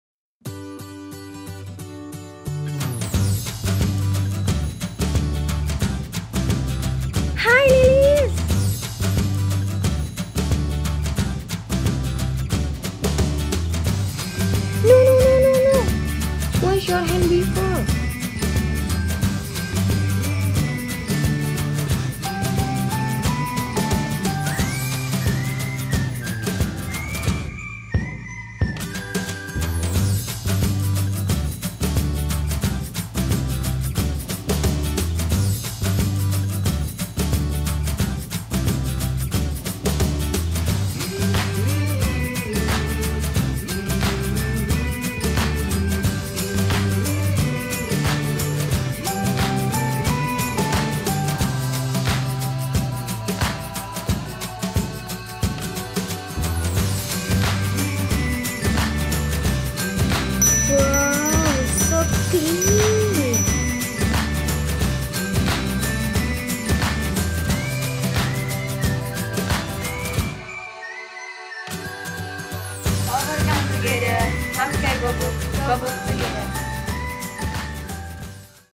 HR: Wash Your Hand Before
Haiii, I'm Dita Anggraeni Yusup. I'm a Honorary Reporters Korea.net 2020 from Indonesia.
I'm meet with lilis and we will eat together but, lilis should wash hands before.
Yeeayyyy. if I'm not home or go to some place, I'm always bring a hand saniitizer.
Bye bye virus
OVERCOME TOGETHER!!! HAMKE GEUKBOK!!!GEUKBOK TOGETHER!!
**) Step by step this wash hand has comfirm WHO, before.
at the date of March 22, I'm as one of regist a Volunteers Covid-19 for Indonesia. And dr. Manullang as Kemenkes (Kementrian Kesehatan RI) teaching wash good and right of hand washing.
Kunjungi juga konten Foto dan Tulisan

Search me on WorldWide browser Dita Anggraeni Yusup or Ditaanggraeniy